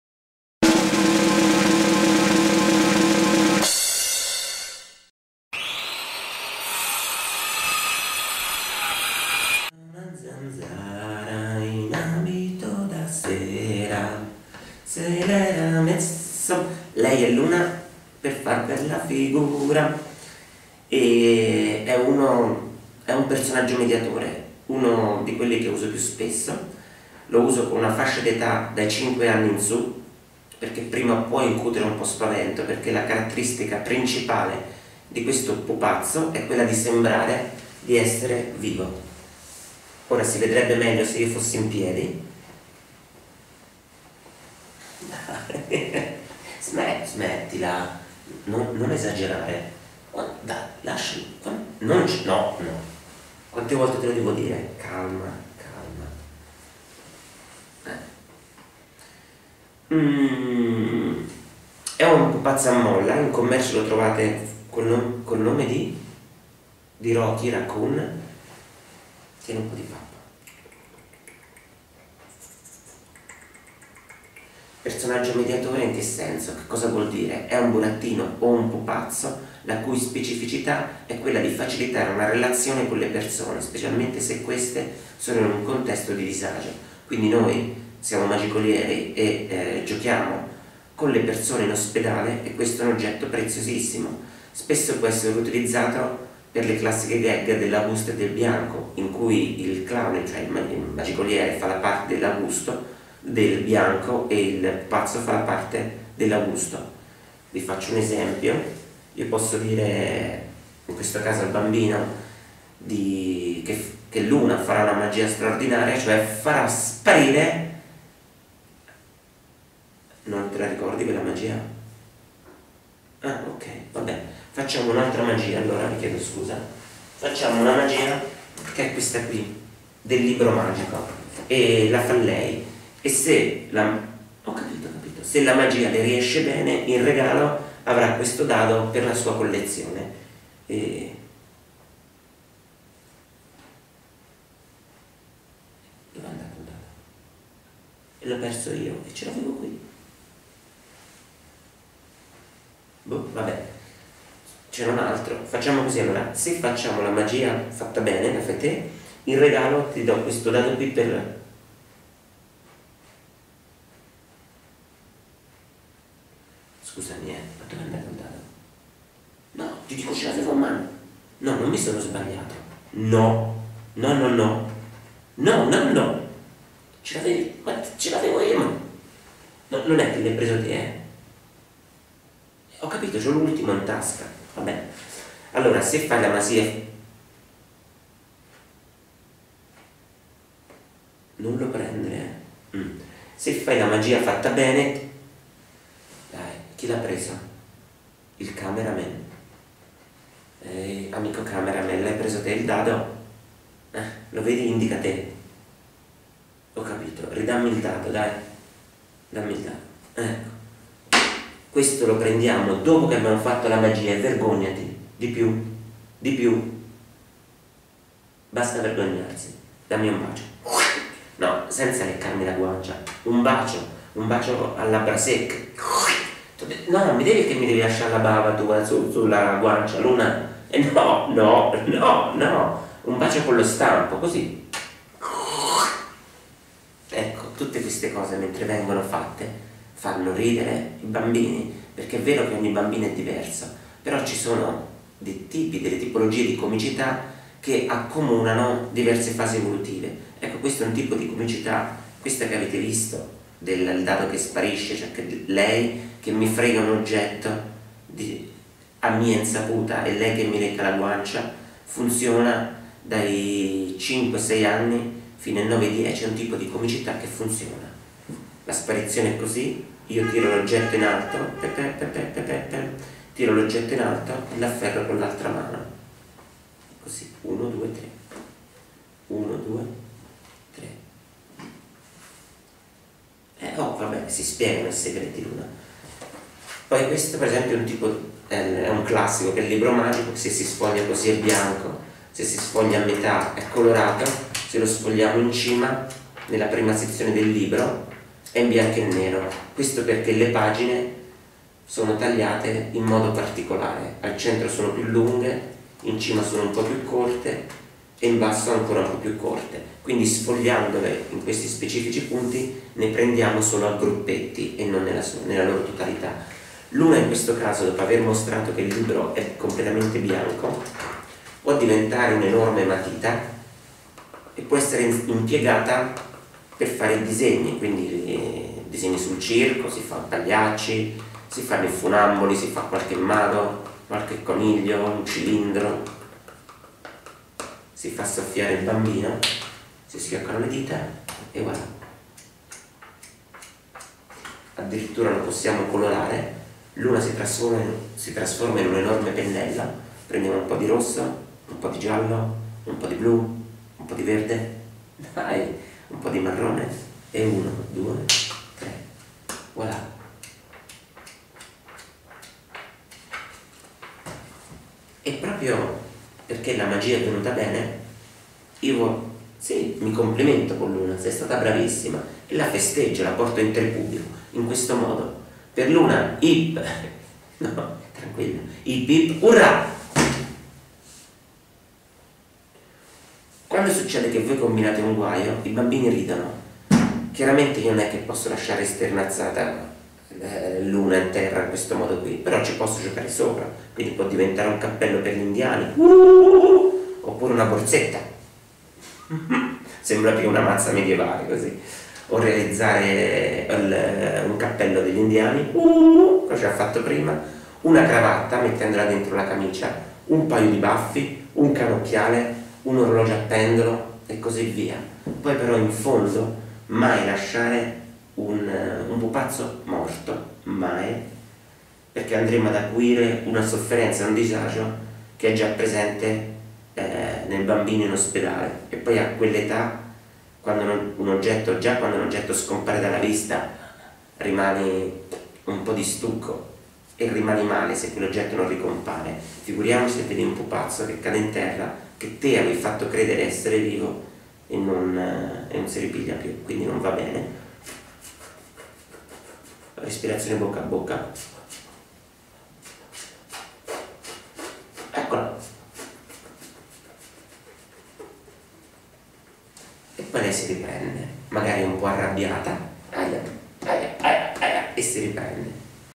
Raccoon - Personaggio mediatore - LUNA
Il personaggio mediatore è un pupazzo che, nelle mani del Magicoliere prende vita.
Viene utilizzato come intermediario nella relazione con il bambino e per questo definito "mediatore".
Il personaggio animato appare meno minaccioso dell'essere umano, è colorato, affabile, dolce e buffo; offre al piccolo la possibilità di esprimersi liberamente, senza sentirsi osservato o interpretato; non deve seguire troppe regole e convenzioni, instaurando con facilità una relazione di gioco.
L'impiego di personaggi mediatori offre grandi possibilità a chi lavora con dei bambini, in particolare quando essi vivono situazioni di stress e disagio.
Appare meno minaccioso dell'essere umano, è colorato, affabile, dolce e buffo; offre al piccolo la possibilità di esprimersi liberamente, senza sentirsi osservato o interpretato; non deve seguire troppe regole e convenzioni, instaurando con facilità una relazione di gioco.Per il piccolo il personaggio mediatore è vivo e non va mai consegnato al bambino. Trovarsi tra le mani un pupazzo floscio ed inespressivo può deludere e portare malinconia.